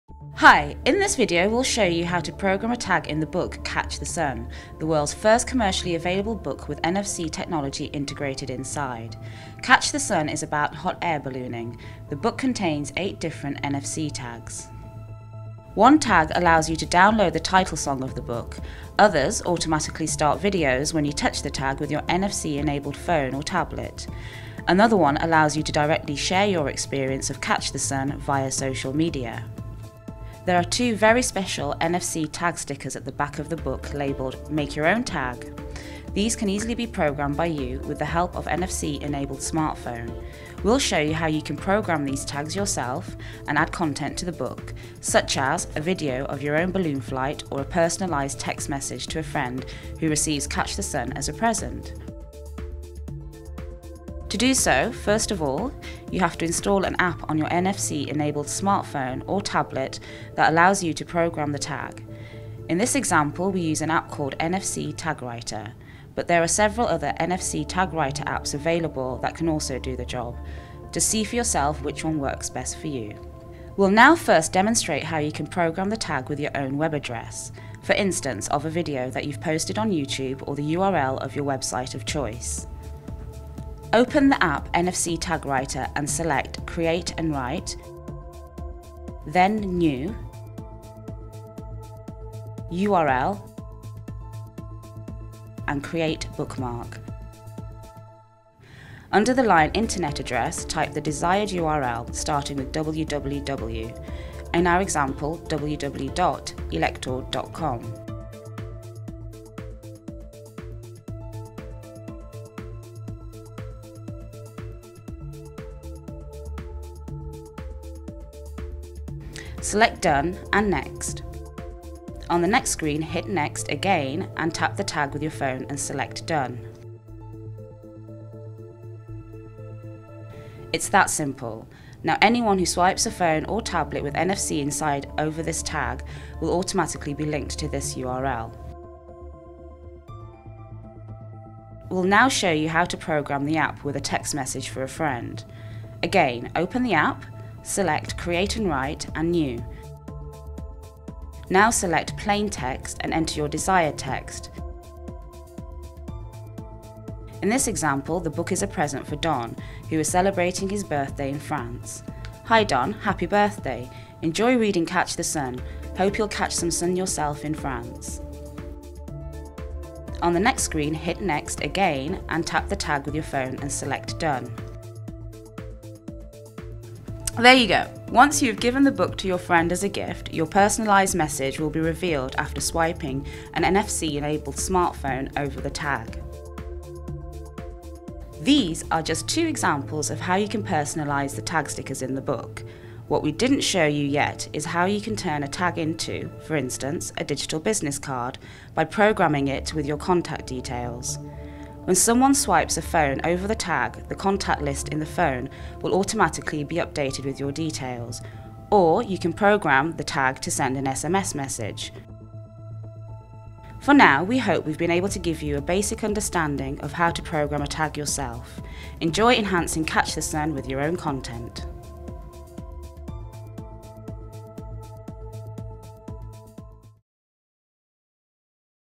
Elektor book - Catch the sun - How-to
CATCH THE SUN provides a fascinating look into the history, science, practice and beauty of ballooning. The full integration of NFC chips in the book is a unique innovation which has not been used before.
When a smartphone or tablet with "NFC-inside" is held against a page with an NFC tag:
movie clips are automatically started | readers can connect to social media | free music can be downloaded | background information on NFC can be accessed | digital content can be added to the book by the reader.